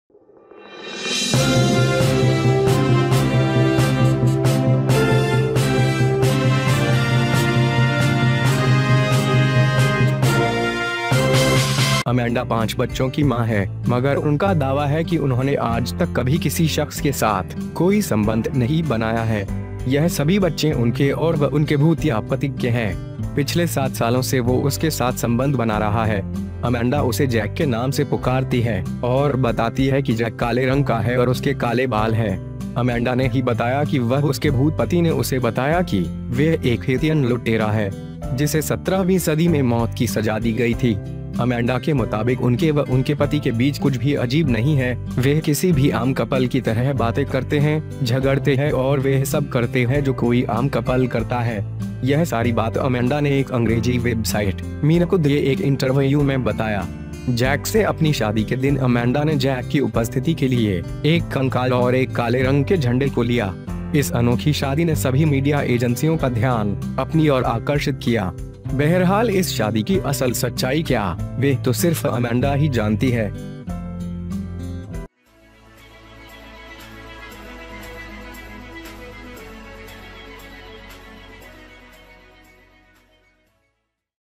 महिला को भूत ने 7 सालों में बनाया 5 बच्चों की मां और अब शादी भी कर ली, देखें शादी की तस्वीरें
Amanda is the mother of 5 children, but she claims that she has never made any connection with any person till date. All these children belong to him and his ghostly husband. For the past seven years, he is having a relationship with her. Amanda calls her through the name of Jack, and tells that Jack is black and has black hair.
Amanda only said that her ghost husband told her that she was a henin robber. Who was sentenced to death in the 17th century. According to Amanda, there is nothing strange between her and her husband, she speaks like any common couple, quarrels, and does everything that a common couple does. All this, Amanda said in an interview given to an English website, Mirror.
On the day of his wedding from Jack, Amanda took a skeleton and a black flag for the presence of Jack. This unique marriage attracted the attention of all the media agencies, and more. However, the real truth of this marriage is that she only knows Amanda.

HJ NEWS !!